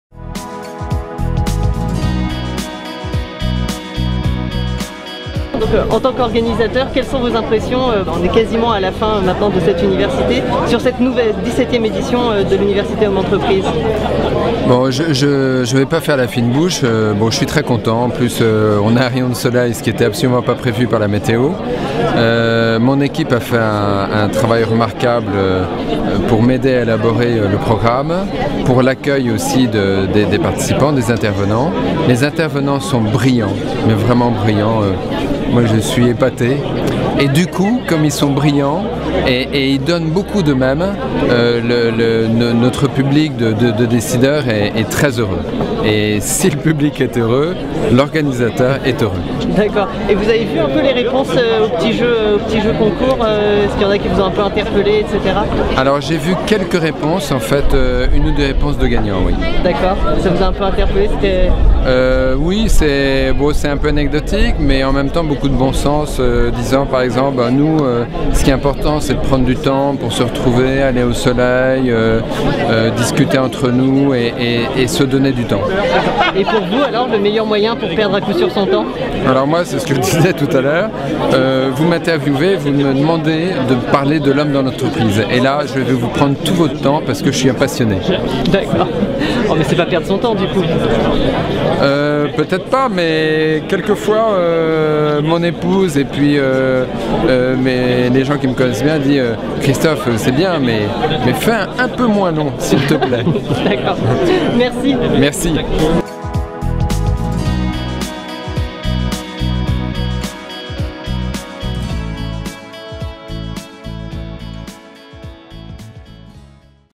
Christophe de la Chaise à l'Université Hommes-Entreprises
Le directeur du CECA nous parle de la 17ème Université Hommes-Entreprises qui a eu lieu les 24 et 25 août 2011 au Château Smith Haut-Laffite.